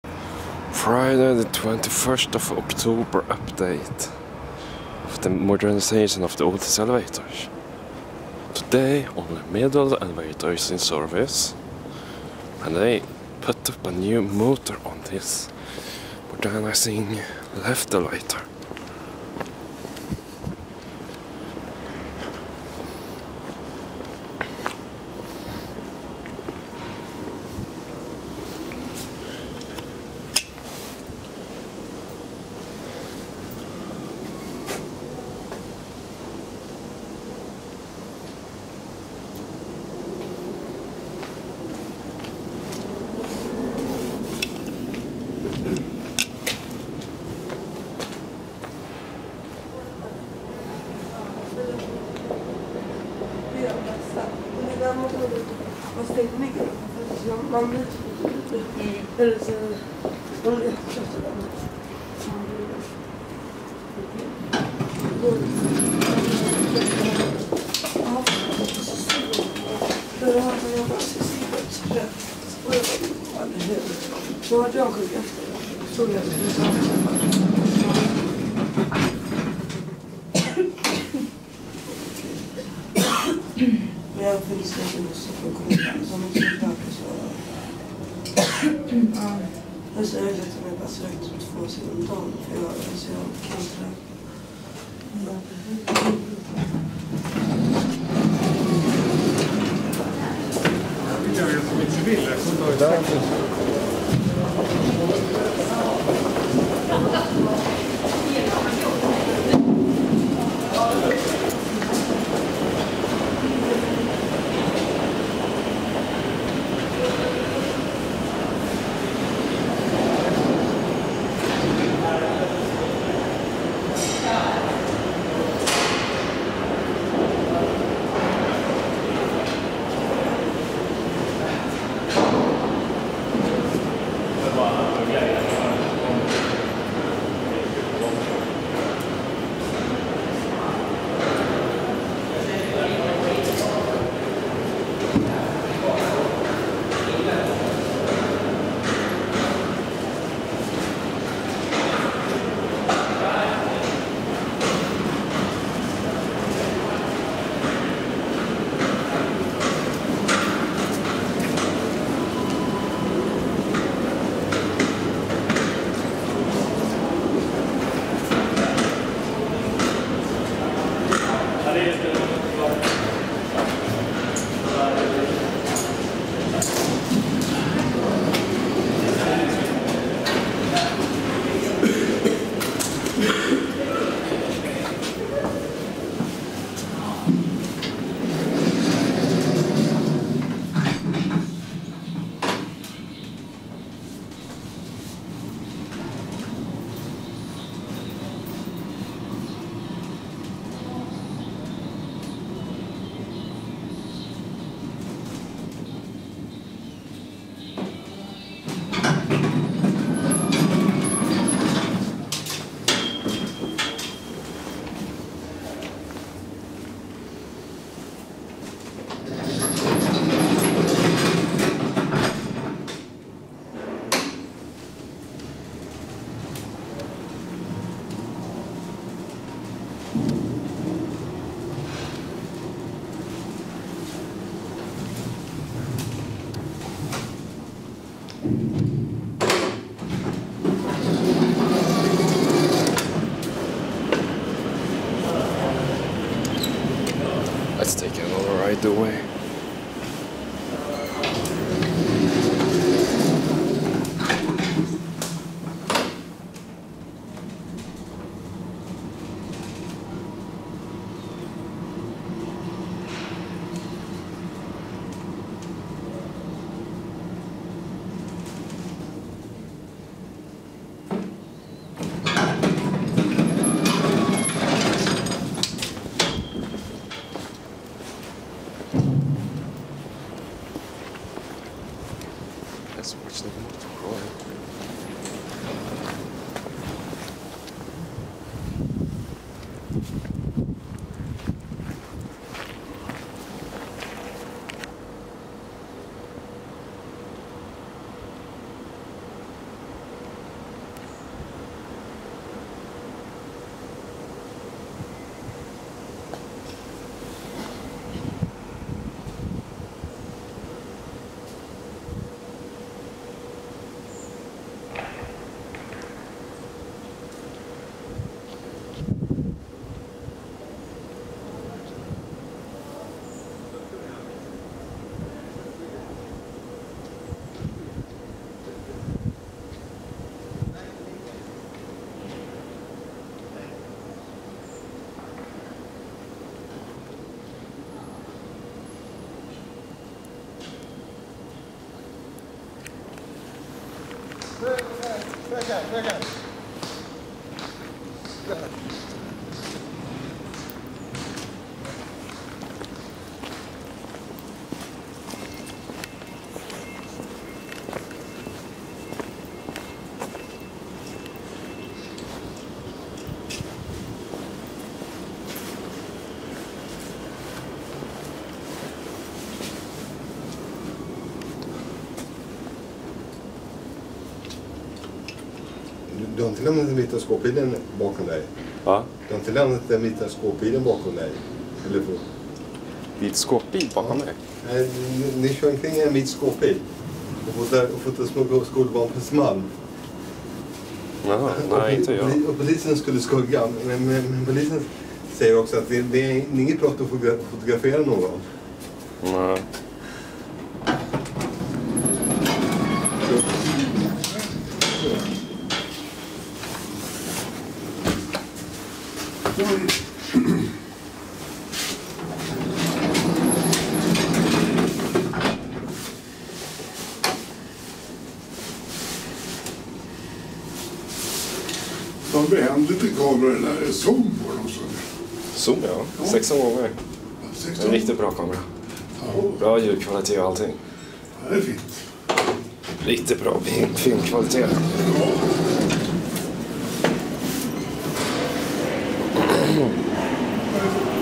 2011-10-21 Flohr-Otis to SMW Elevator Replacement Update @ Gärdet Metro Station
Update of the elevators being replaced in Gärdet Metro Station recorded in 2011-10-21.
The shaft doors has been removed!
From: 1967 Flohr-Otis Traction Elevators
To: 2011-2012 SMW Traction Elevators
GÄR 21261
GÄR 21262
GÄR 21263